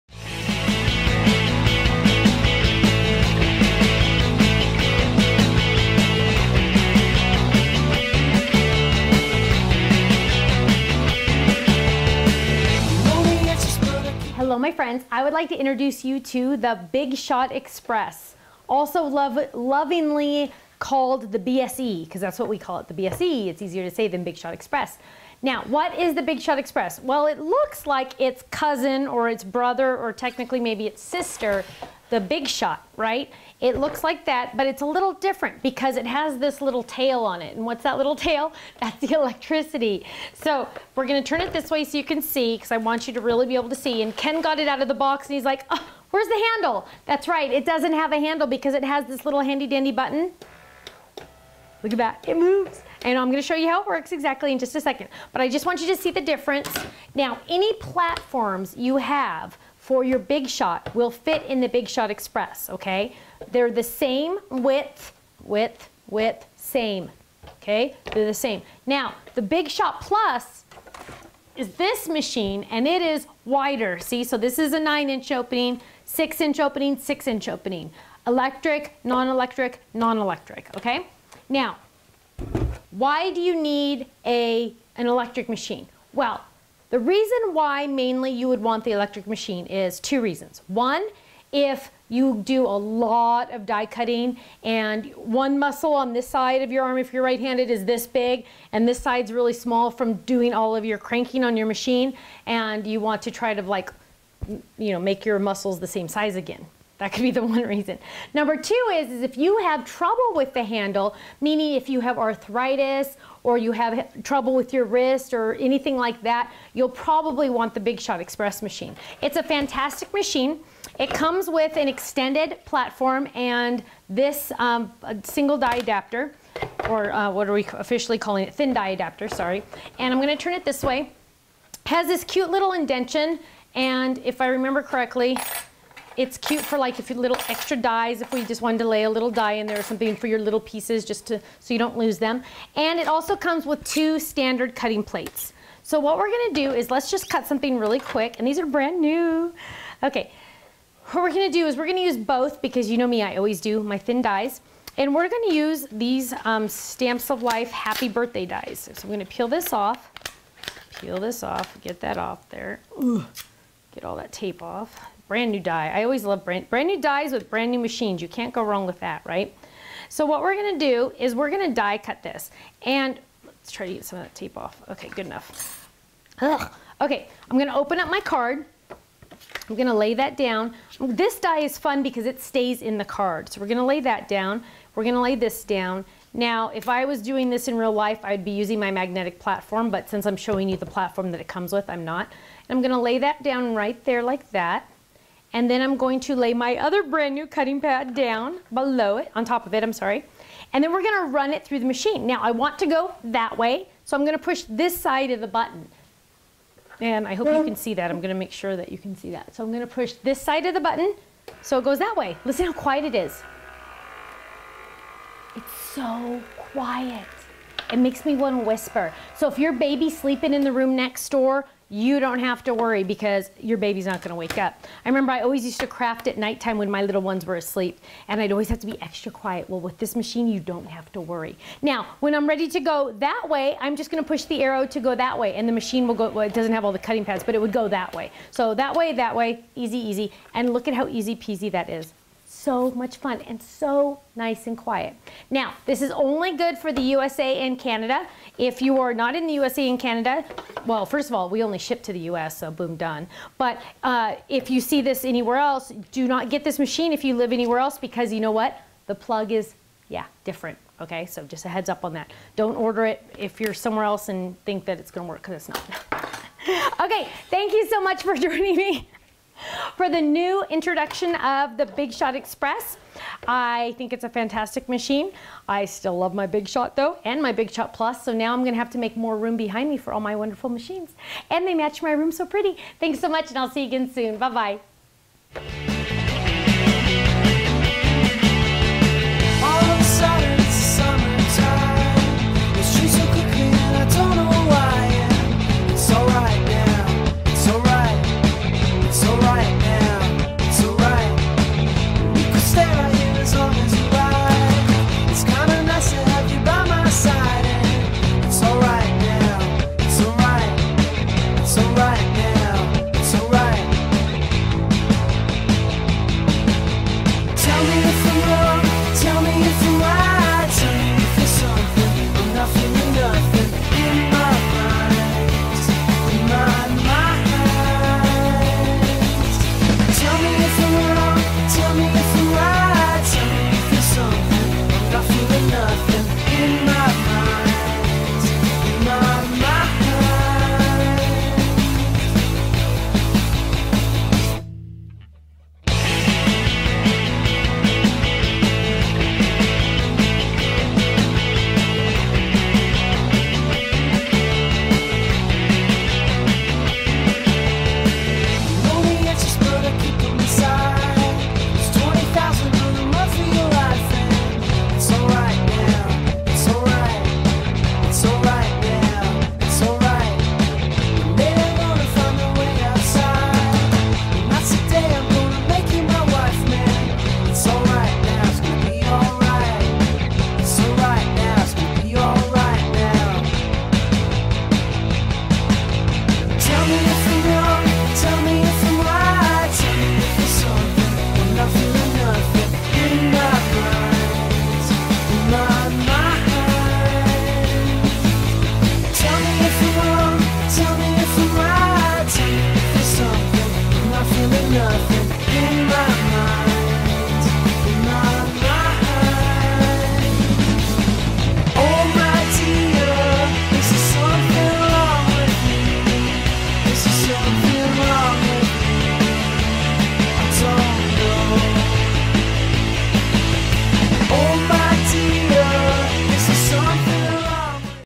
The Stamps of Life: Sizzix Big Shot Express
The Stamps of Life and Stephanie Barnard would like to share this new die cutting and embossing machine from Sizzix called the Big Shot Express! This is a powered Big Shot that makes die cutting so easy! You'll want to watch this video.